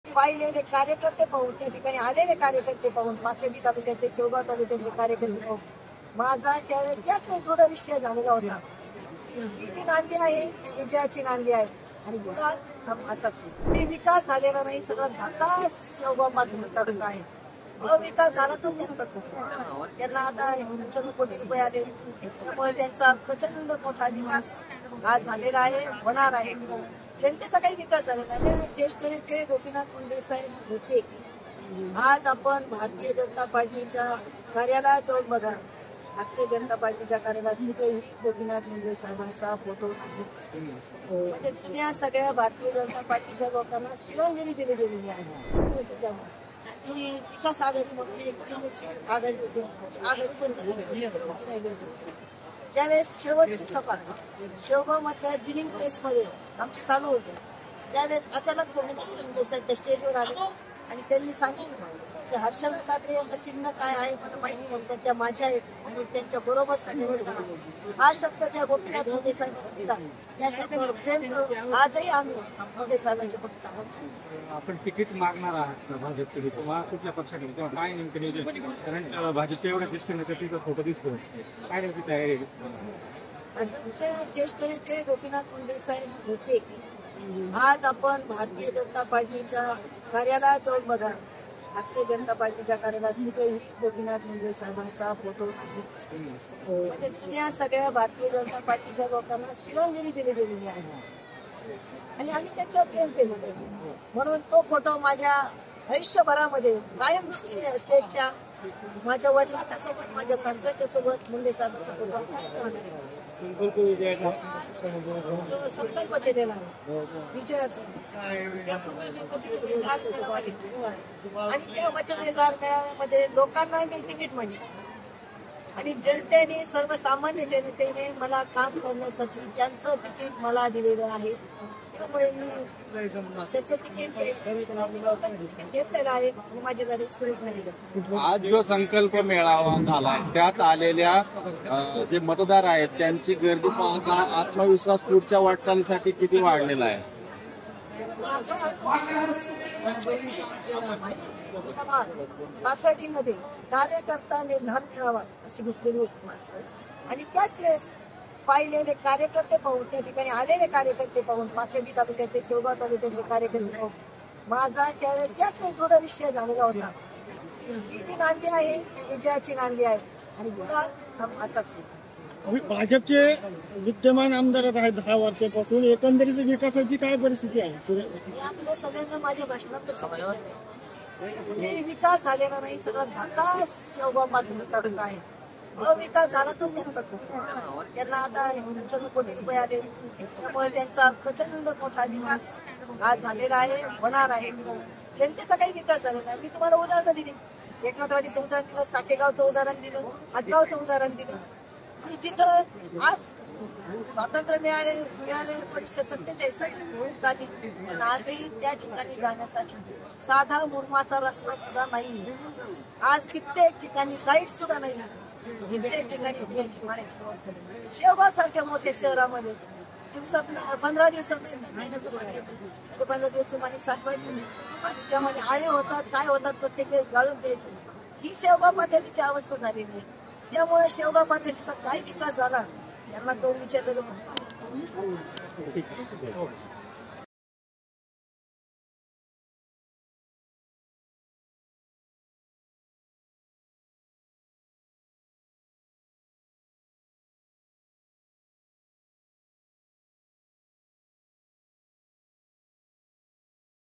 Harshada Kakde : 'गुलाल माझाच असणार', हर्षदा काकडेंनी रणशिंग फुंकलं | LetsUpp Marathi
शेवगाव-पाथर्डी विधानसभा मतदार संघाच्या आमदार मोनिका राजळे यांच्यावर हर्षदा काकडे यांनी जोरदार हल्लाबोल केला.